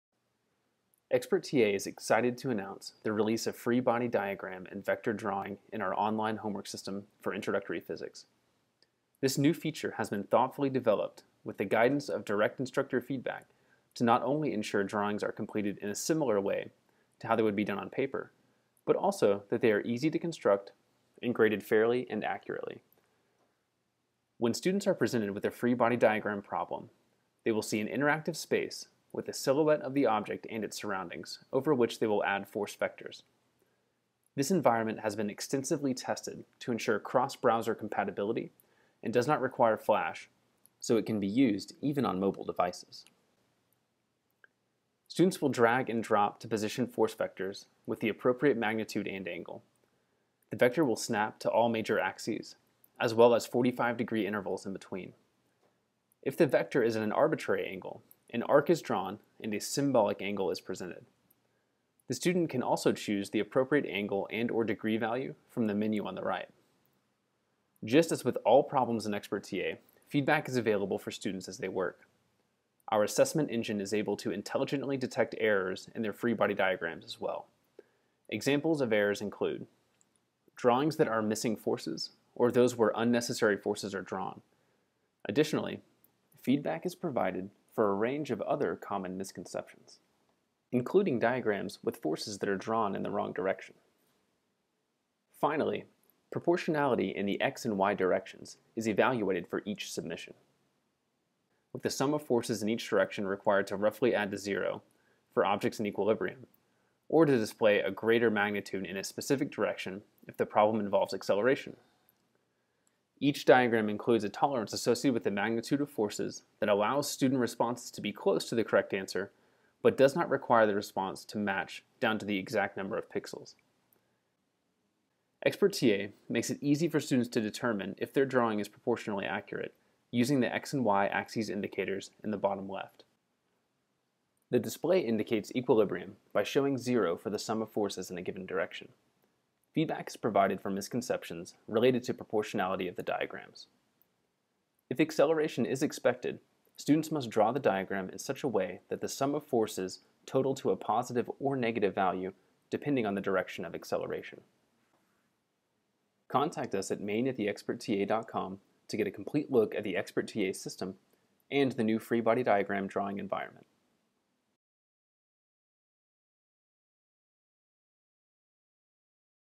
Expert TA Interactive Free Body Diagrams
An overview of the new interactive free body diagram drawing environment in Expert TA's online homework system for introductory Physics courses.